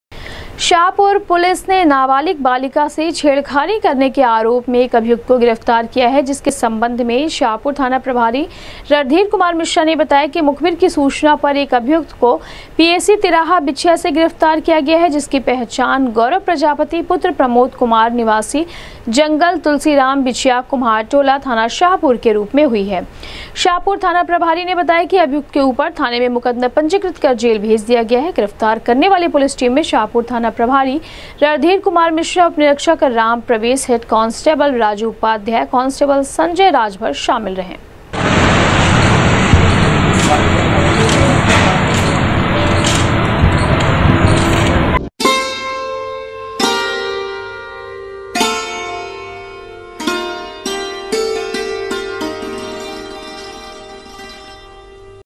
नाबालिग बालिका के साथ छेड़खानी करने के आरोप में शाहपुर पुलिस ने एक अभियुक्त को किया गिरफ्तार,
नाबालिग बालिका के साथ छेड़खानी करने के आरोप में शाहपुर पुलिस ने एक अभियुक्त को किया गिरफ्तार,भेजा जेल।।